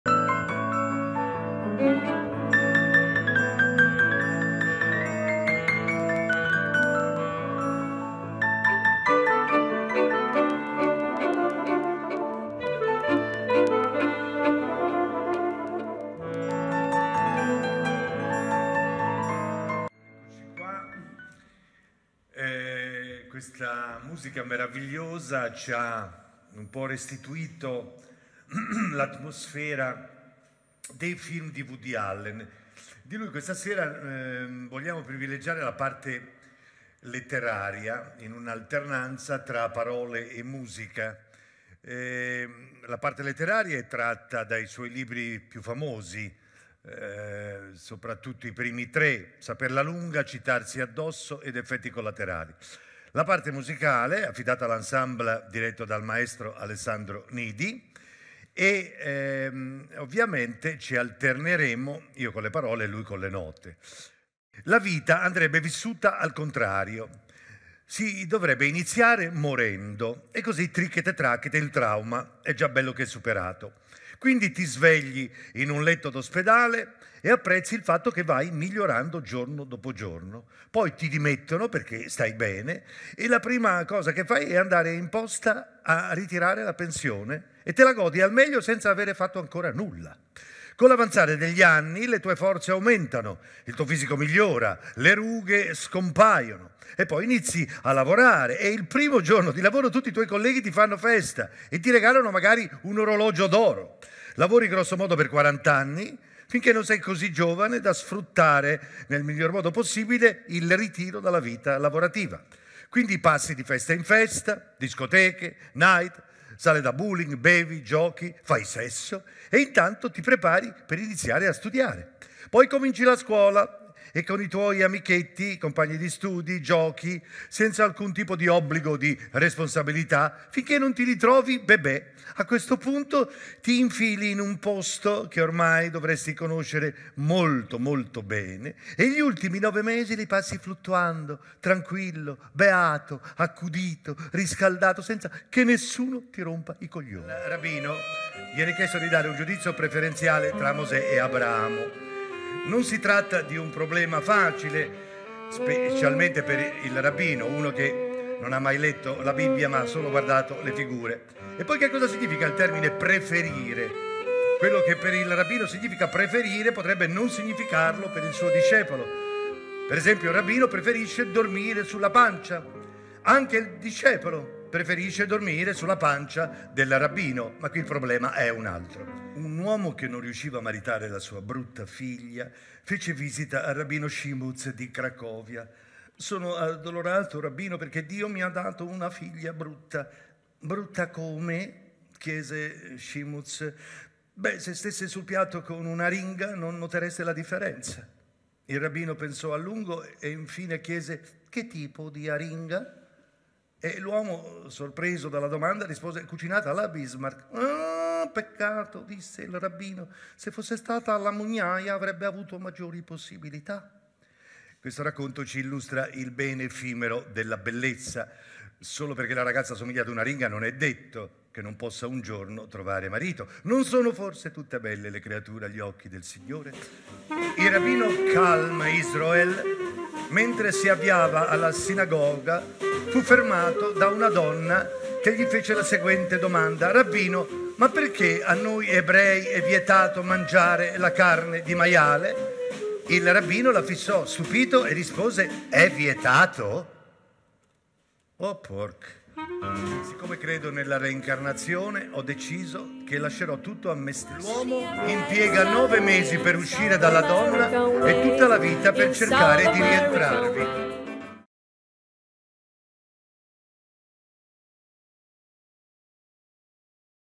Tullio Solenghi in Dio è morto e neanch'io mi sento tanto bene (Trailer ufficiale)
DIO É MORTO E NEANCH’IO MI SENTO TANTO BENE
Le parole del genio Woody Allen immerse nella sua musica
di e con
TULLIO SOLENGHI
e con NIDI ENSEMBLE

Tullio Solenghi ci diletta con la lettura di alcuni esilaranti brani tratti dai libri di Woody Allen, coniugandoli con le musiche che hanno caratterizzato i suoi film più significativi, eseguite dal maestro Alessandro Nidi e dal suo Ensemble.
Si passerà così dai “Racconti Hassidici” alla parodia delle Sacre Scritture tratti da “Saperla Lunga” allo spassoso “Bestiario” tratto da “Citarsi Addosso”, intervallati da brani di George Gershwin, Tommy Dorsey, Dave Brubeck, con uno speciale omaggio al mentore di Woody, il sommo “Graucho Marx”, evocato dalla musica Klezmer.
Una serata in cui, in rapida carrellata, si alterneranno suoni e voci, musica e racconto in un’alternanza di primi piani a comporre un “montaggio” divertente e ipnotico.
produzione International Music and Arts